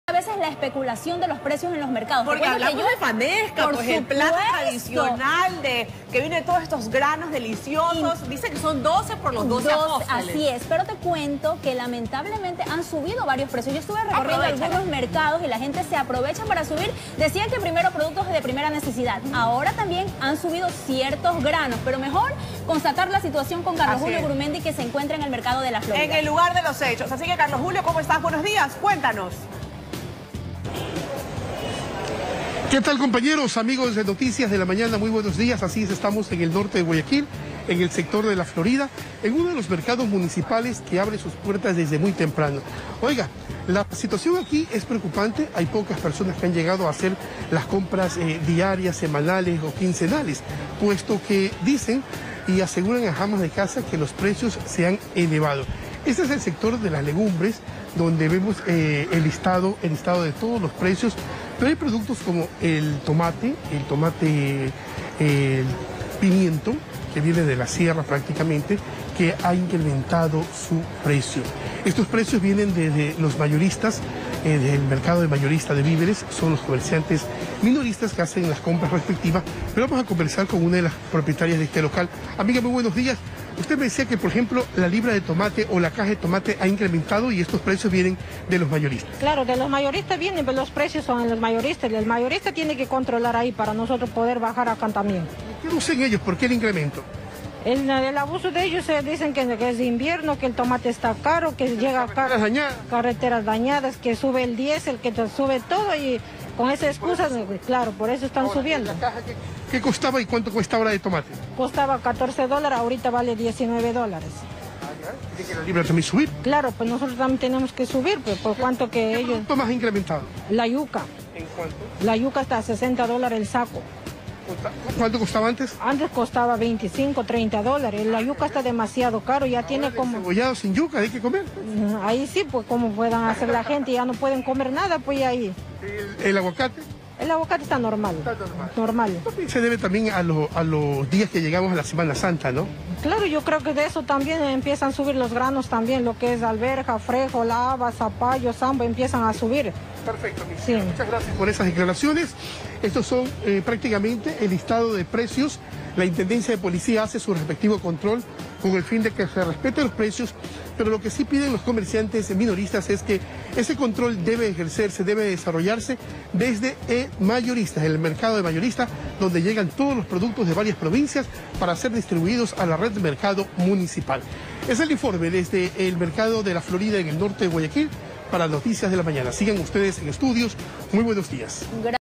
Mercados y tiendas elevan los precios de varios productos
Comerciantes reportan que la subida es porque los mayoristas venden muy caro por la crisis.
Varios productos elevaron sus precios por la crisis y por la Semana Santa.